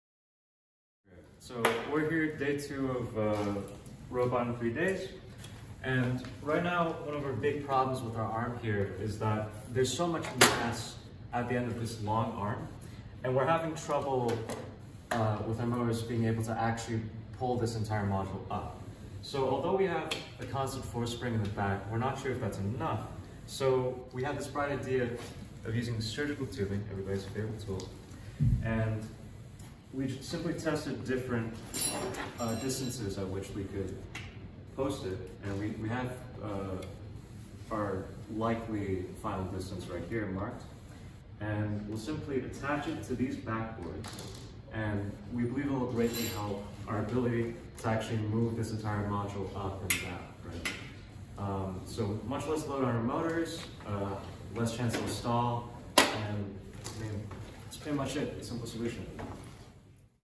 Ri3D @ Purdue Day 2: Surgical Tubing Counterbalance
David is back to present a problem the lifter subteam faced, and their simple yet unconventional solution to said problem.